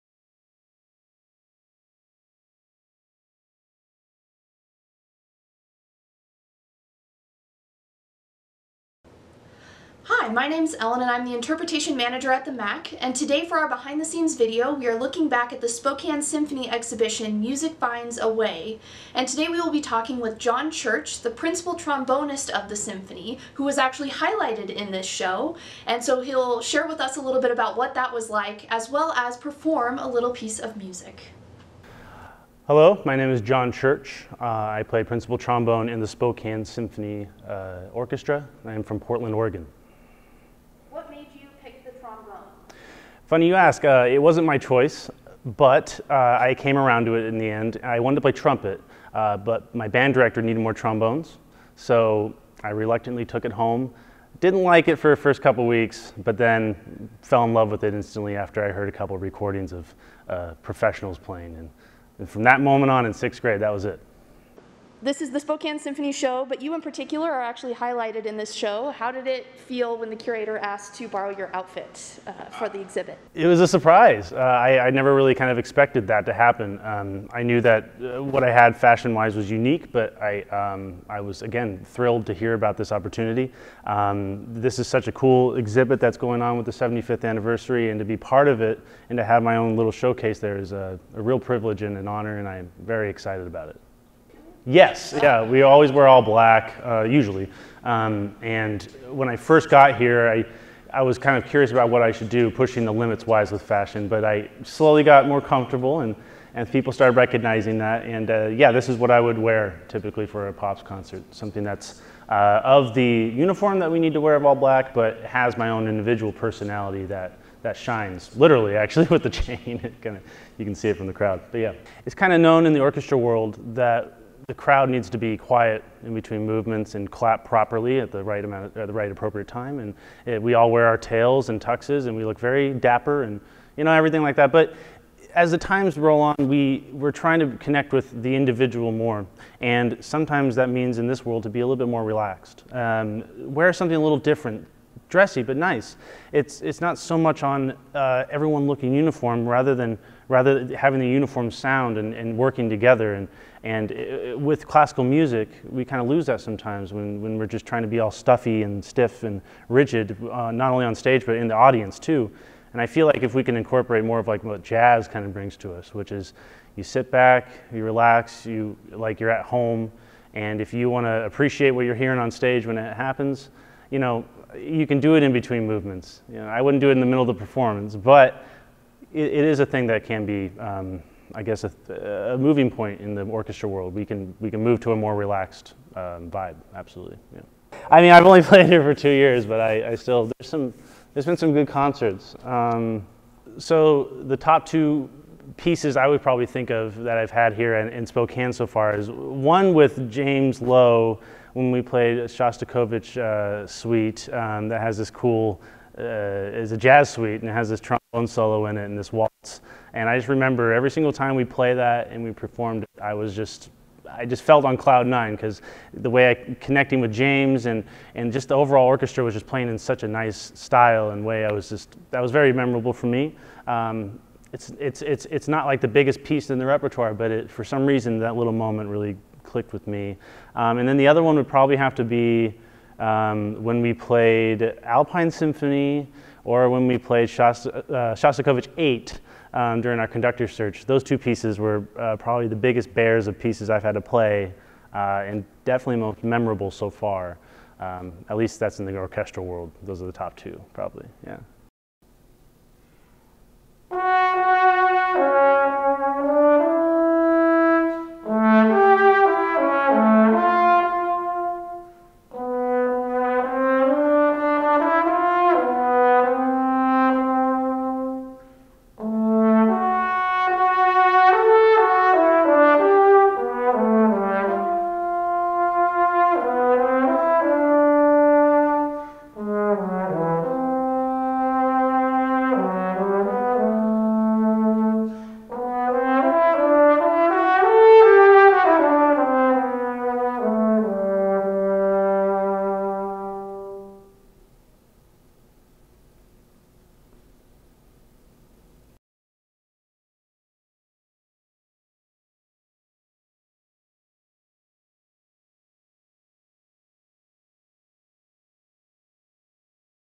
Behind the Scenes: Symphony Exhibit Highlight with John Church
Join our Interpretation Manager in the galleries with John Church, the Principal Trombonist of the Spokane Symphony. John shares with us the importance of finding your individual flare, and helps us welcome in the first video of the new year with a little music!